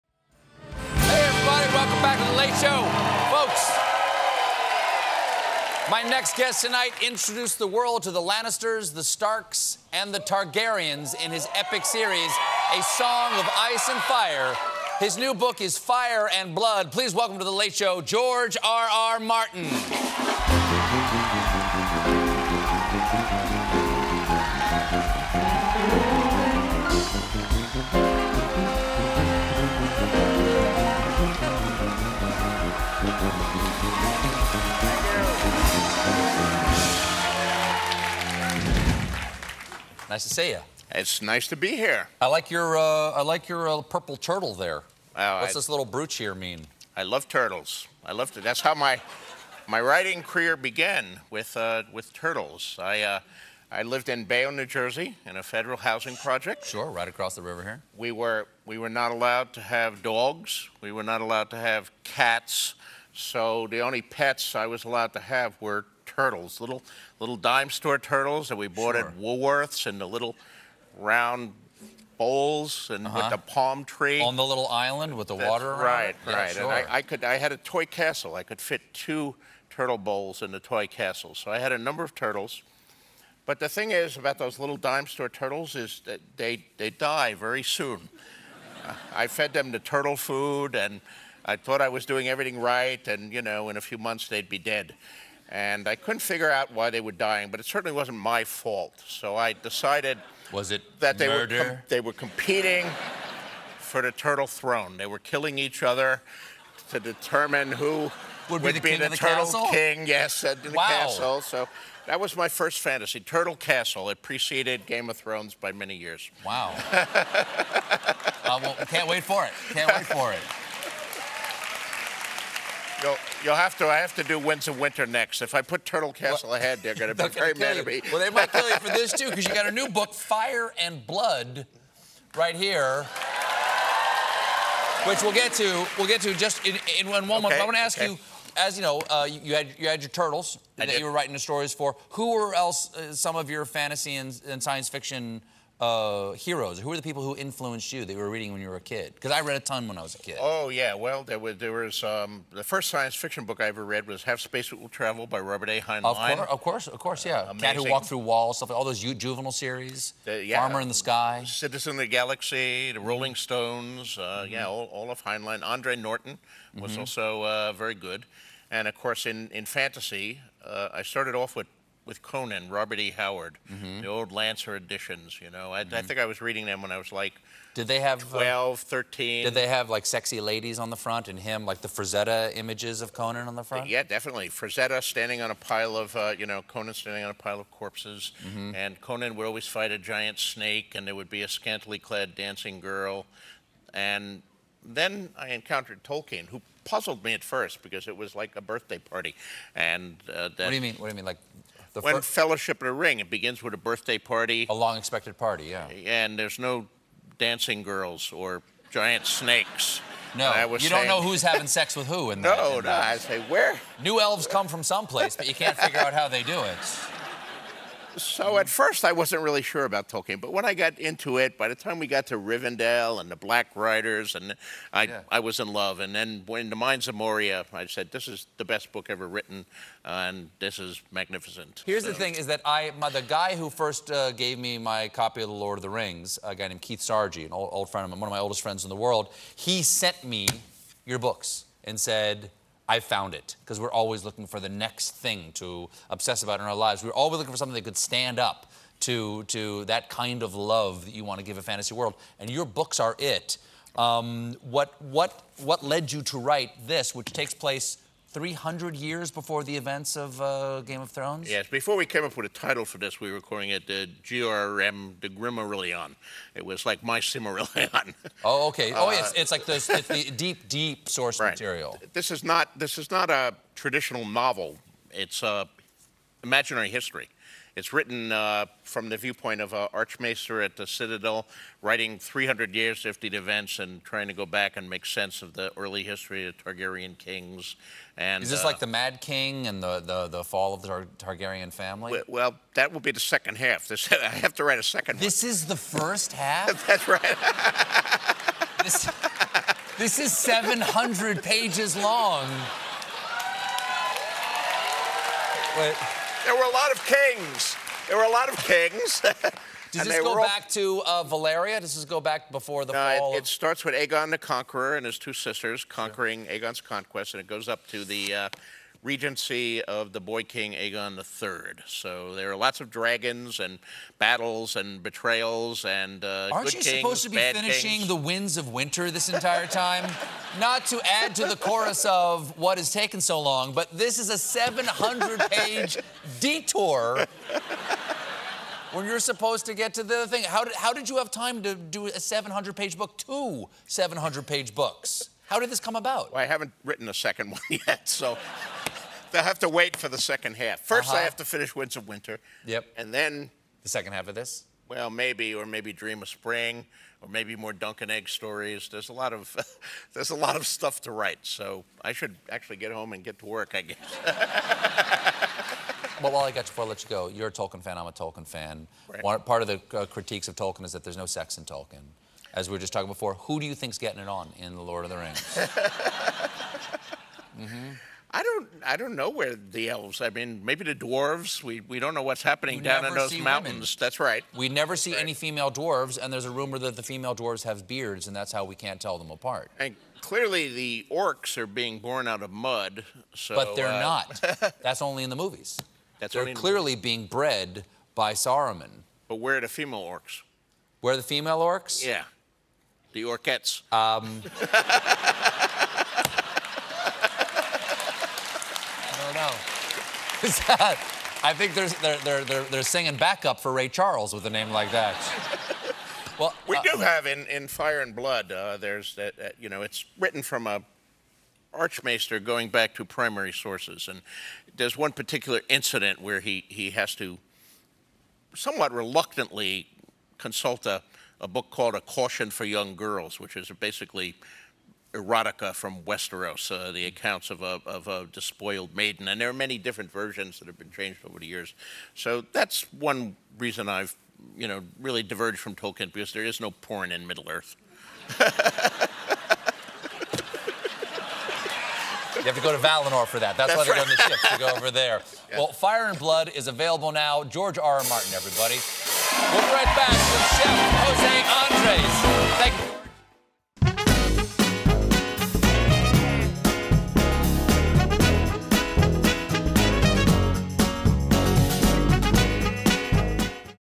George R. R. Martin's Earliest Inspiration Of All
'Fire and Blood' author George R. R. Martin shares his writing inspirations, which range from Tolkien to his childhood pet turtles.